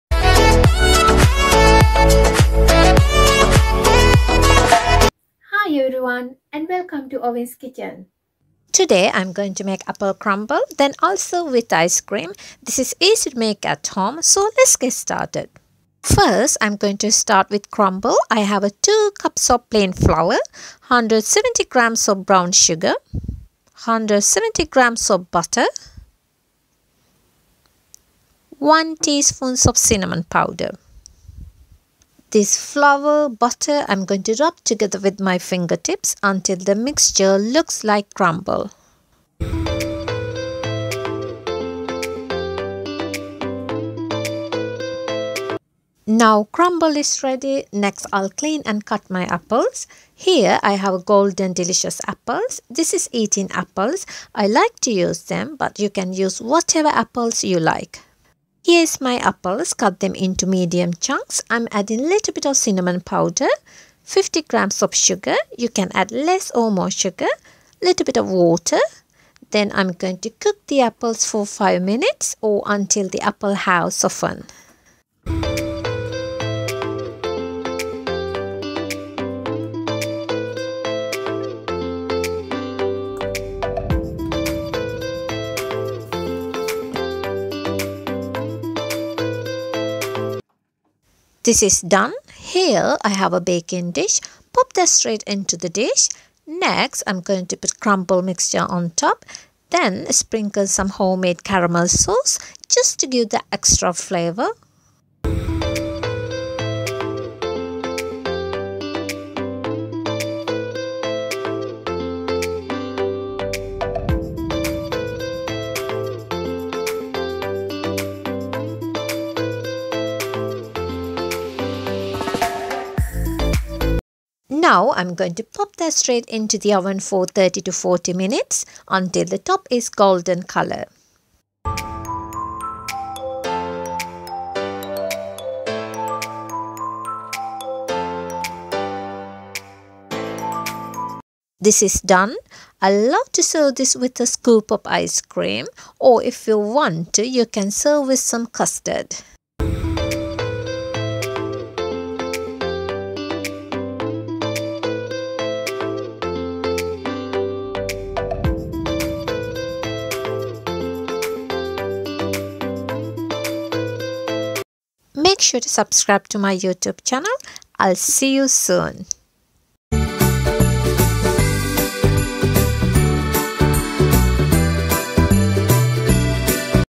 Apple crumble 🍏 Owen's Kitchen
Homemade apple crumble recipe.🍏
so delicious! and it is easy to make at home.